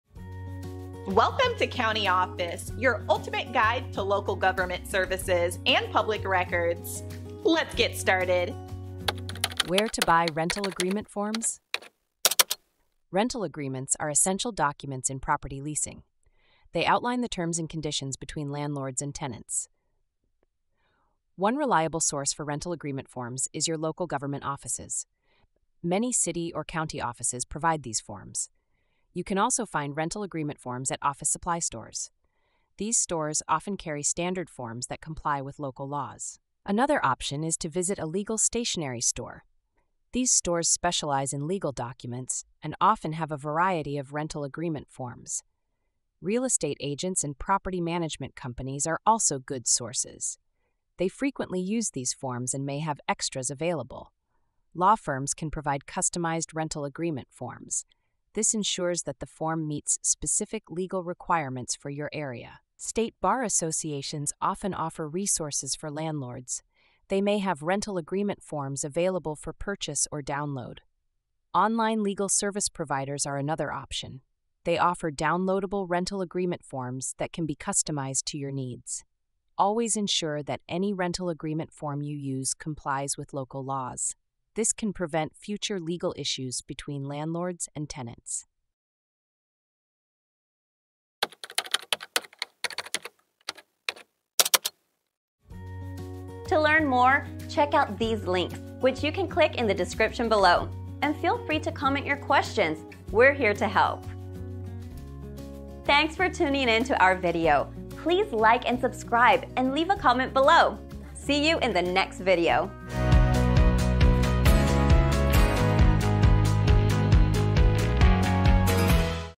Where To Buy Rental Agreement Forms? - CountyOffice.org
Where To Buy Rental Agreement Forms? Are you in need of rental agreement forms for property leasing? In this video, we explore various reliable sources where you can purchase or download rental agreement forms to ensure a smooth landlord-tenant relationship. From local government offices to legal stationery stores, office supply stores, real estate agents, property management companies, law firms, state bar associations, and online legal service providers, we cover all the options available to you. It's crucial to use rental agreement forms that comply with local laws to avoid any legal issues down the line. Stay informed and equipped with the right resources to protect your rights as a landlord or tenant.